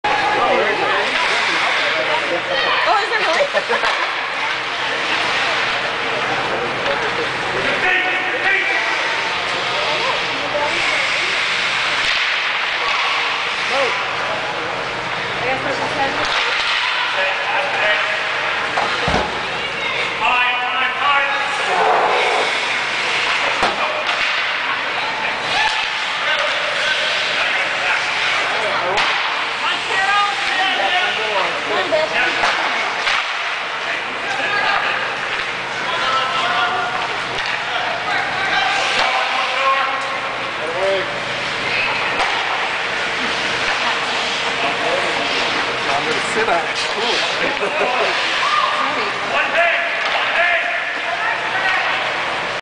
Wisconsin Badgers Womens C Club Team South Bend
Action from the January 2007 Tournament in South Bend Indiana